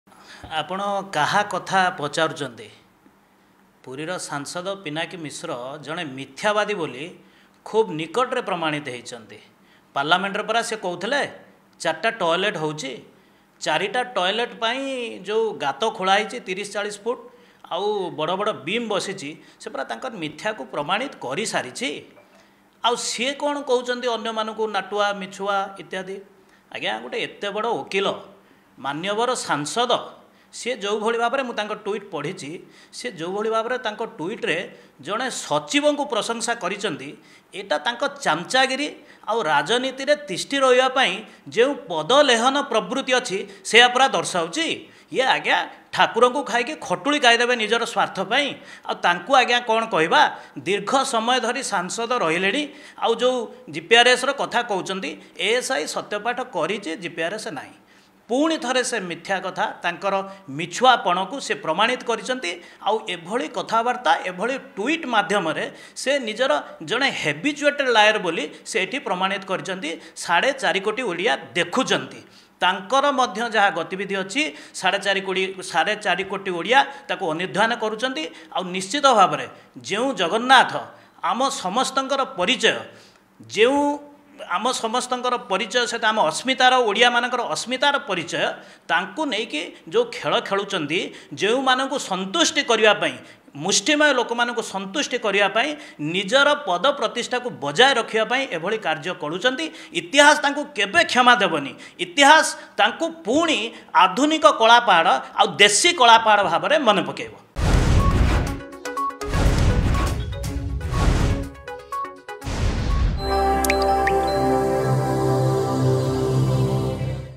ପିନାକି ମିଶ୍ର ମିଥ୍ୟବାଦୀ ବୋଲି ପ୍ରମାଣିତ ହୋଇଛନ୍ତି
ପୁରୀର ସାଂସଦ ପିନାକି ମିଶ୍ର ମିଥ୍ୟବାଦୀ ବୋଲି ଖୁବ୍ ନିକଟରେ ପ୍ରମାଣିତ ହୋଇଛି । ପାର୍ଲାମେଣ୍ଟରେ କହିଥିବା କଥା ଏବଂ ଶୌଚାଳୟ ପାଇଁ ଖୋଳାଯାଇଥିବା ଗାତ ପିନାକି ମିଶ୍ରଙ୍କର ମିଥ୍ୟାକୁ ପ୍ରମାଣ କରିସାଛି ।